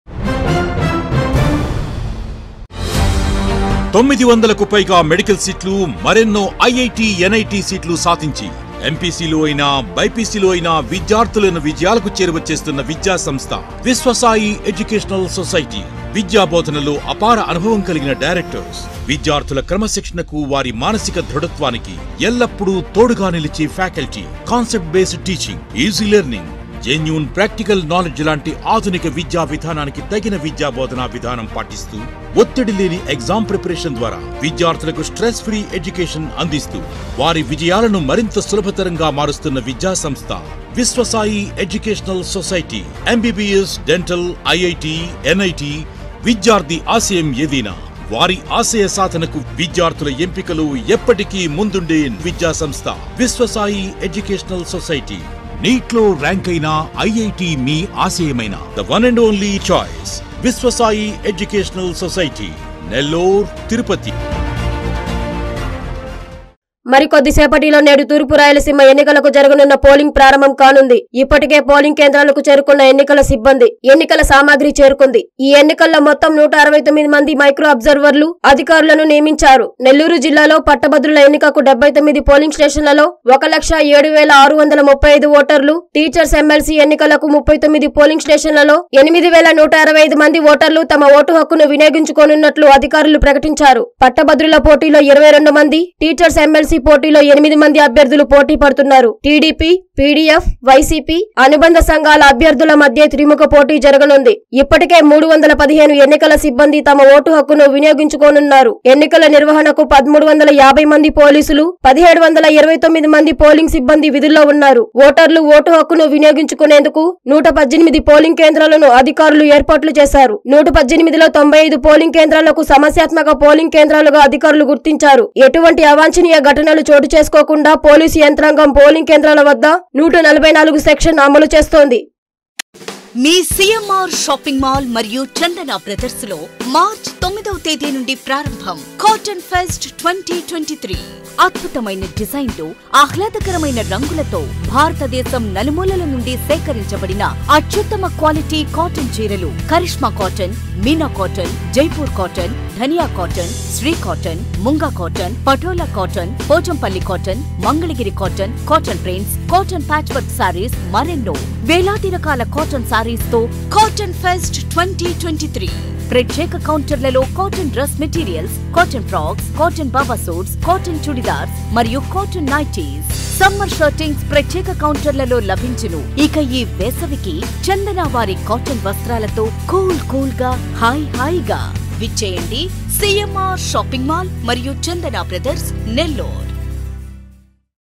నేడే పోలింగ్‌... పోలింగ్ కేంద్రాల‌కు చేరుకున్న ఎన్నిక‌ల సిబ్బంది
నేడే పోలింగ్‌...
- పోలింగ్ కేంద్రాల‌కు చేరుకున్న ఎన్నిక‌ల సిబ్బంది
- 169 మంది మైక్రో అబ్జ‌ర్వ‌ర్లు నియామ‌కం
- ప‌ట్ట‌భ‌ద్రుల ఎన్నిక‌కు 1,07,635 మంది ఓట‌ర్లు
- టీచ‌ర్ల ఎన్నిక‌కు 8,165 మంది ఓట‌ర్లు
పోలింగ్ కేంద్రాల వ‌ద్ద 144 సెక్ష‌న్‌